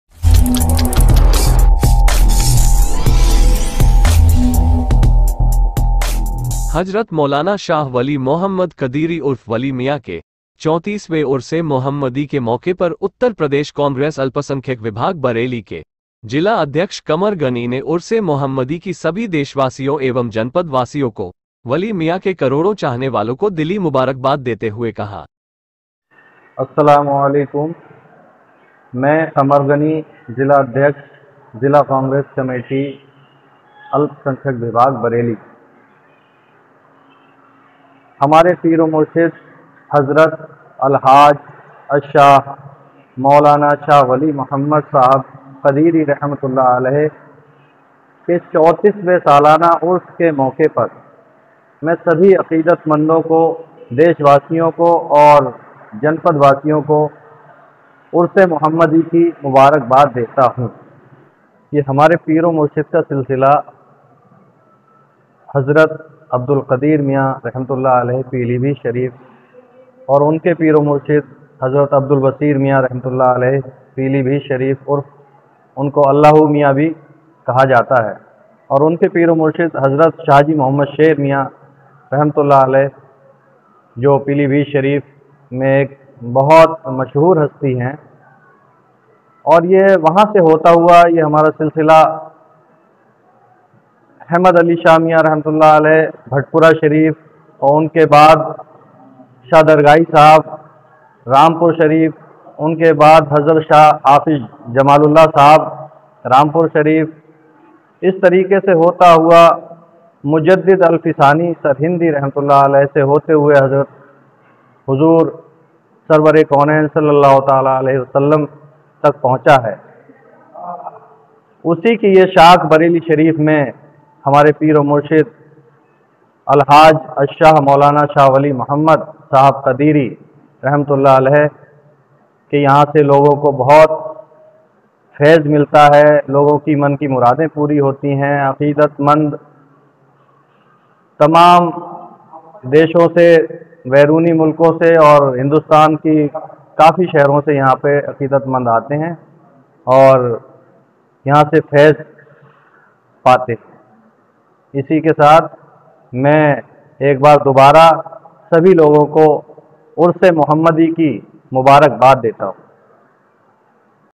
बरेली, कांग्रेस नेता क़मर ग़नी ने 34 वें उर्स ए मौहम्मद उर्फ वली मियां की मुबारकबाद दी
कांग्रेस नेता क़मर ग़नी ने 34 वें उर्स ए मौहम्मद उर्फ वली मियां की मुबारकबाद दी